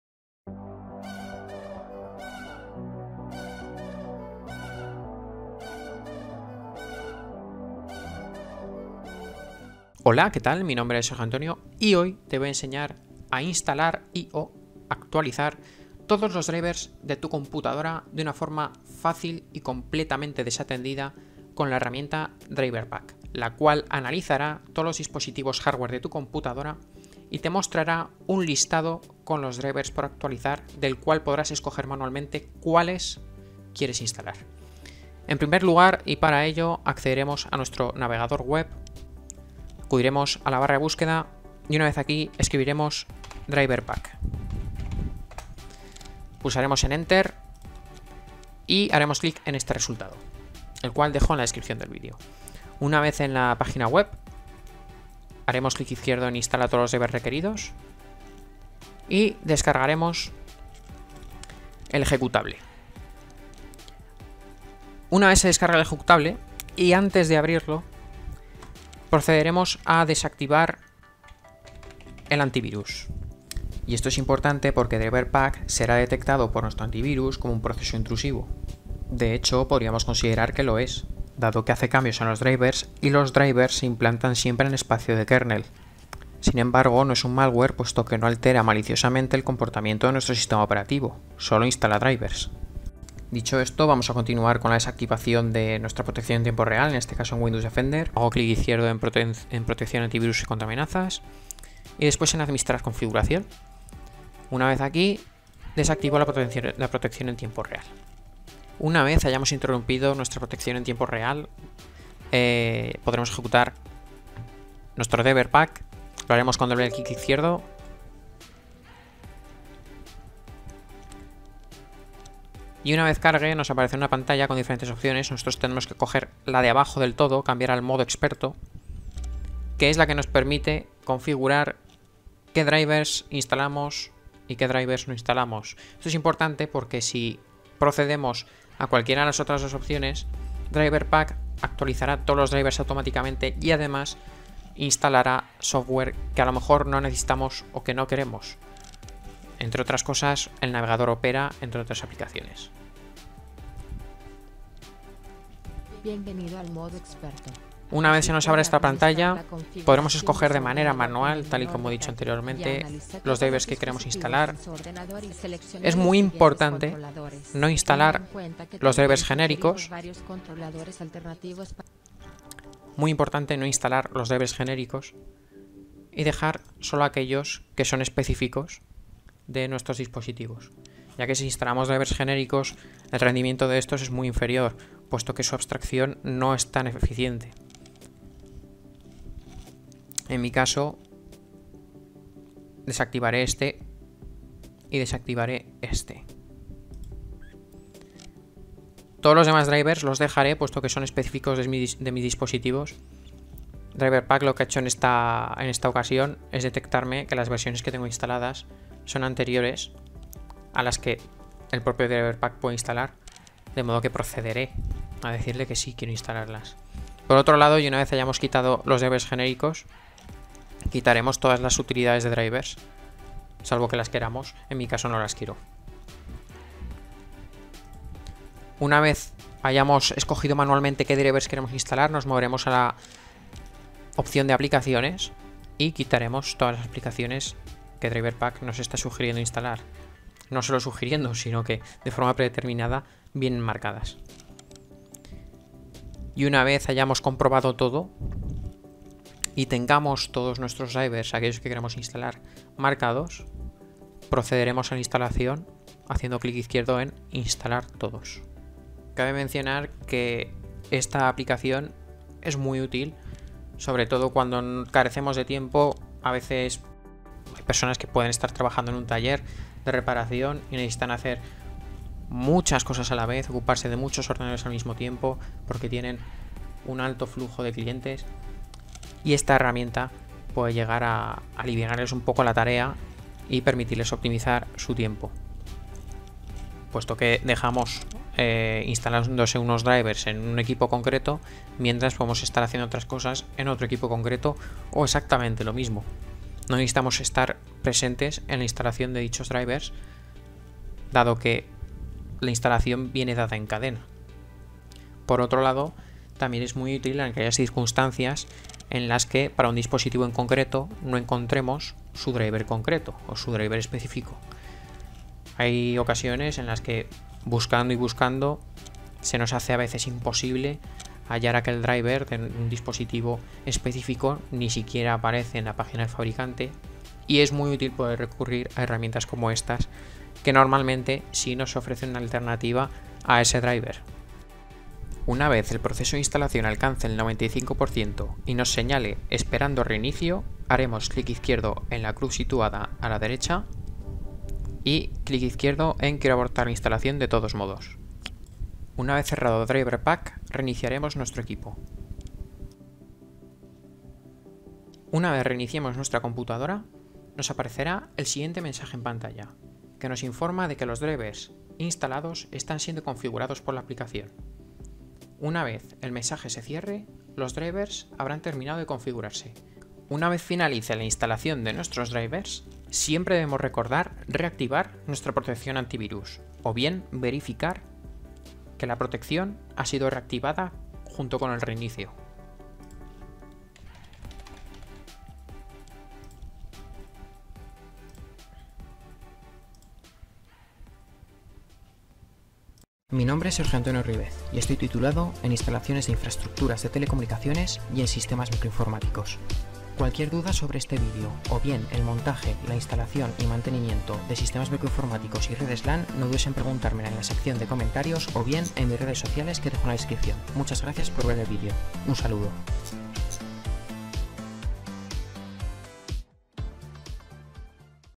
Instalar todos los drivers a la vez con Driver Pack | Instalación desatendida de controladores
En este vídeo explico como instalar y/o actualizar de manera fácil , rápida, segura y desatendida todos los drivers de nuestro pc con driverpack, evitando tener que descargar los drivers desde la página del fabricante e instalarlos uno a uno y ahorrándonos tiempo.